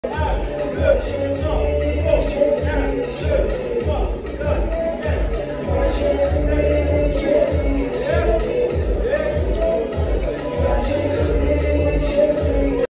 Kizomba workshop (Rdc🇨🇩 Goma)
Buja kiz presents kizomba workshop in DRC🇨🇩 GOMA
 c'est un bon moment avec les autres danseurs des clubs différents

choreographer:
Sefth_Chris

for dance class / private dance class